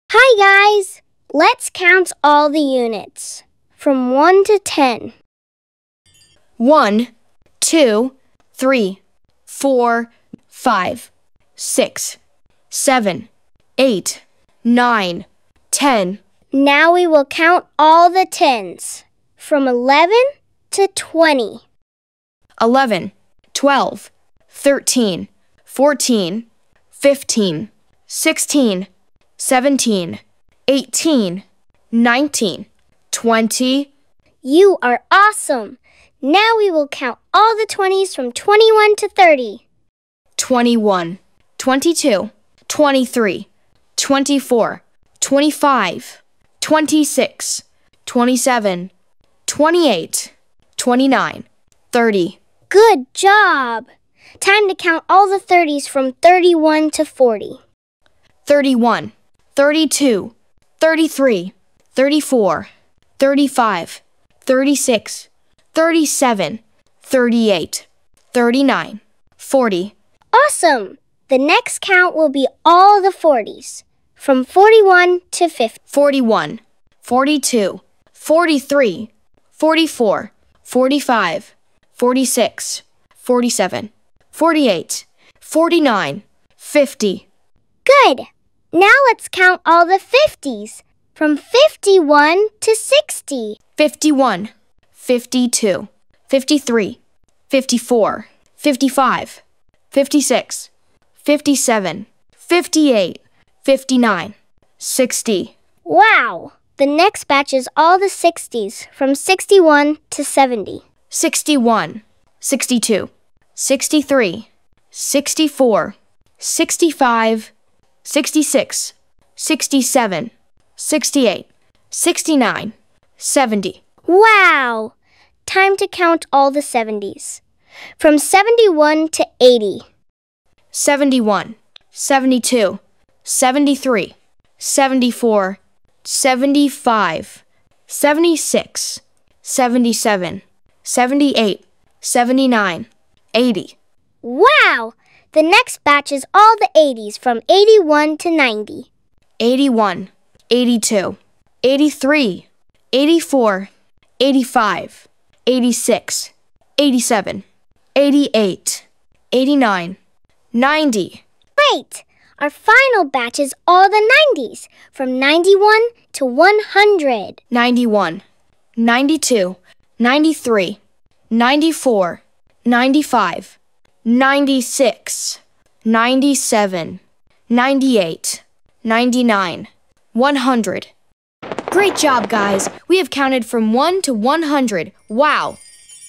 Counting Numbers from 1 to 100 | Counting in Batches of 10
Hi guys, let's count from 1 to 100. We will do that in batches of 10. A very fun way to learn to count and take breaks. Get ready guys!. countingin10s counting1to100 #100